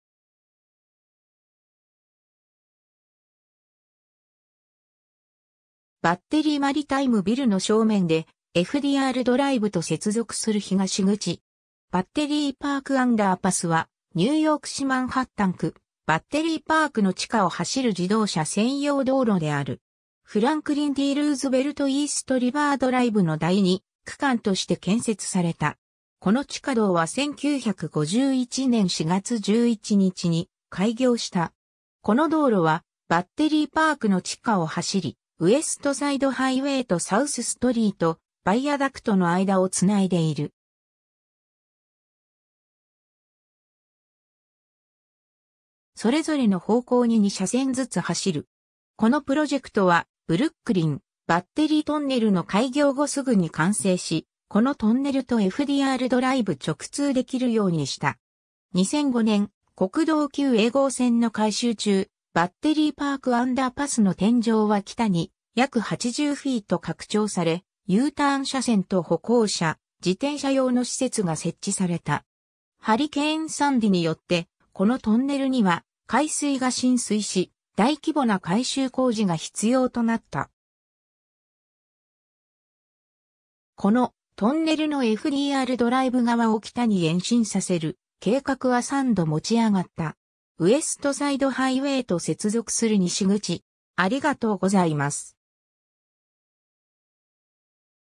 バッテリーパークアンダーパス
バッテリー・マリタイム・ビルの正面でFDRドライブと接続する東口。
 バッテリーパークアンダーパス (Battery Park Underpass) は、ニューヨーク市マンハッタン区バッテリー・パークの地下を走る自動車専用道路である。
フランクリン・D・ルーズベルト・イースト・リバー・ドライブの第二区間として建設された。
 この地下道は1951年4月11日に開業した。
この道路はバッテリー・パークの地下を走り、ウエスト・サイド・ハイウェイとサウス・ストリート・バイアダクトの間をつないでいる。
それぞれの方向に二車線ずつ走る。
このプロジェクトはブルックリン-バッテリートンネルの開業後すぐに完成し、このトンネルとFDRドライブ直通できるようにした。
 2005年、国道9A号線の改修中、バッテリーパークアンダーパスの天井は北に約80フィート拡張され、Uターン車線と歩行者・自転車用の施設が設置された。
 ハリケーン・サンディによって、このトンネルには海水が浸水し、大規模な改修工事が必要となった。
 このトンネルのFDRドライブ側を北に延伸させる計画は三度持ち上がった。
 ウエスト・サイド・ハイウェイと接続する西口